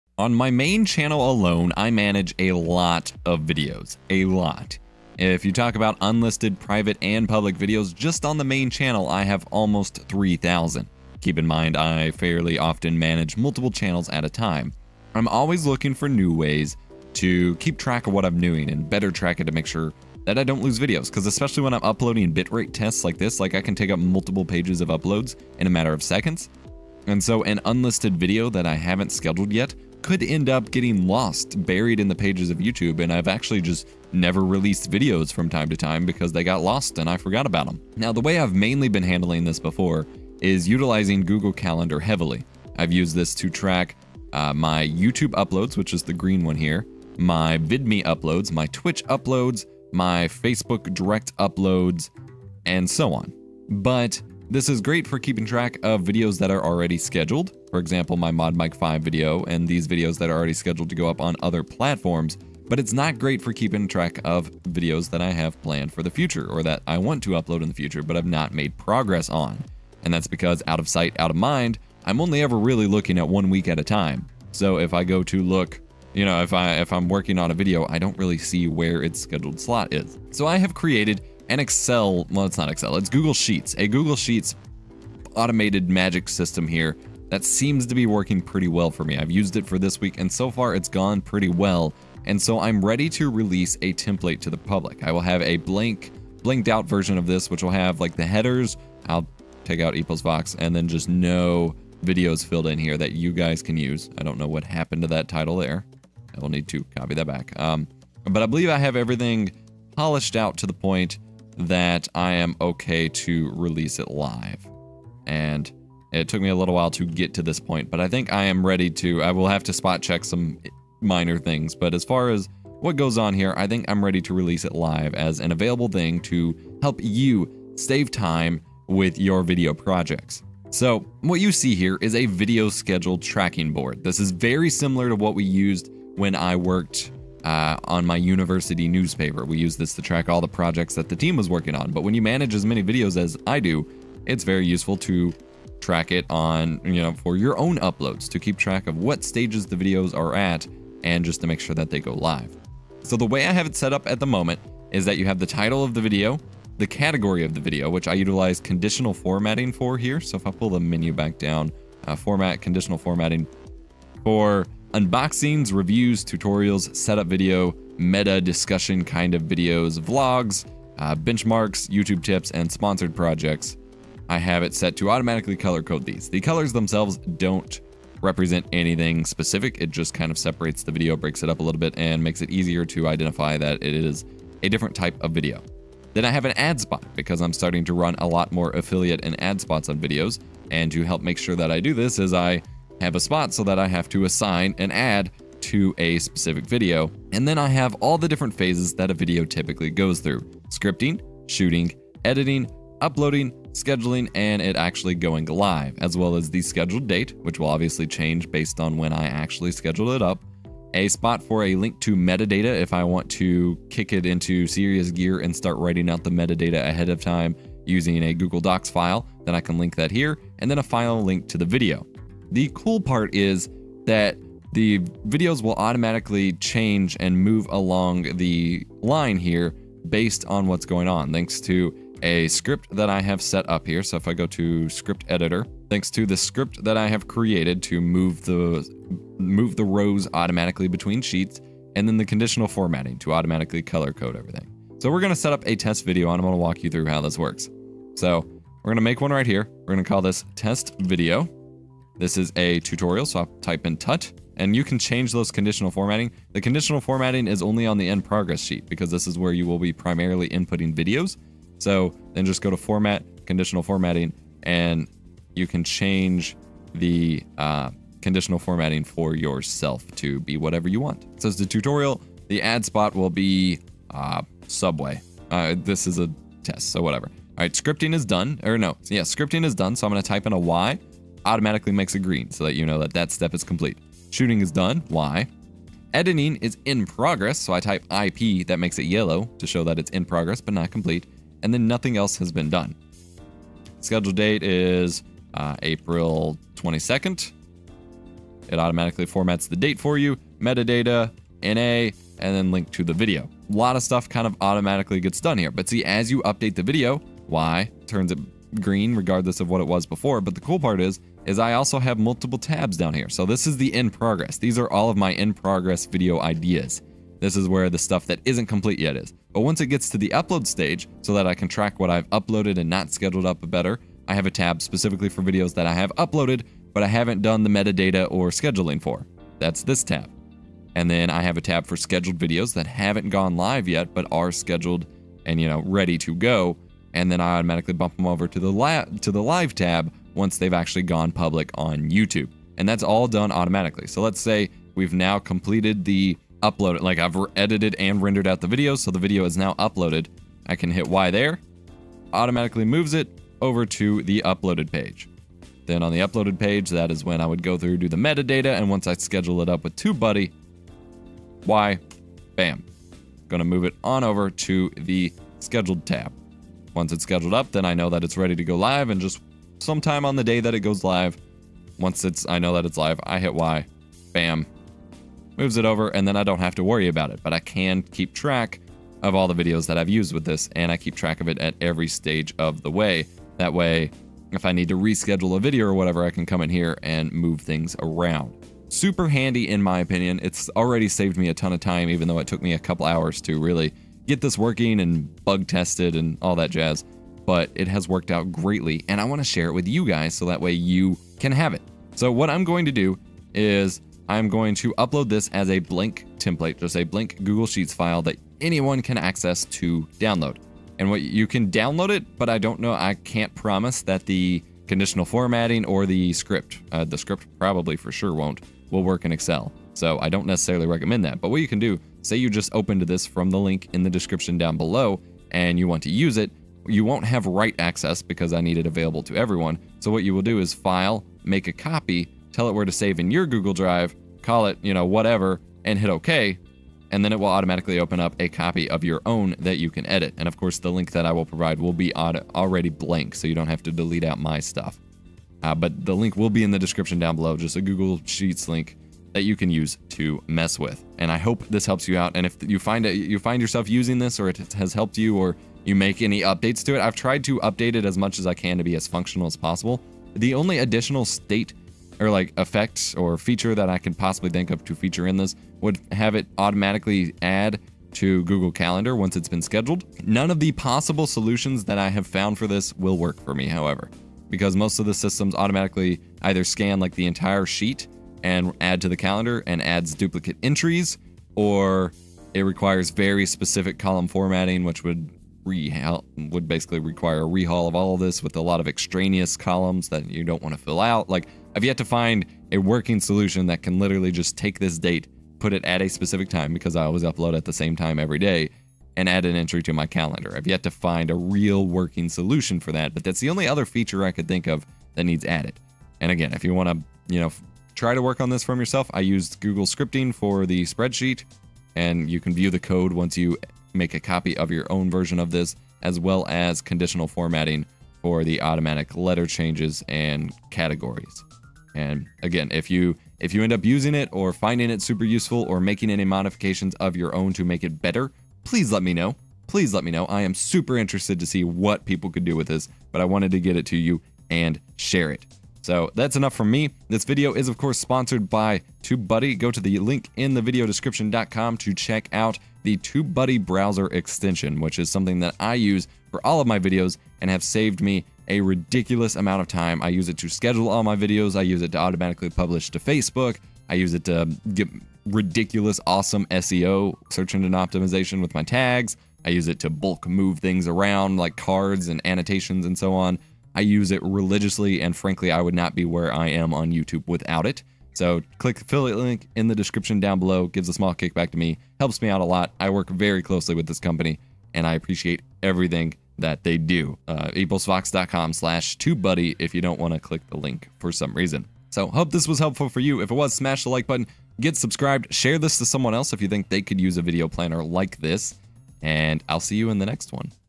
Save Time & Plan Videos! - FREE Time-Saving Tool Release
Whenever I make or find something that increases my efficiency and makes my workflow better or saves me time, I try my best to share it with you guys. I have made a video planner template in Google Sheets using Scripting and Conditional Formatting. This allows you to track video progress and see when they're scheduled and when they go live.

Access on Google Sheets
(Go to File - Make a Copy to save it and make your own.)

Let me know if you come up with any cool mods for this sheet!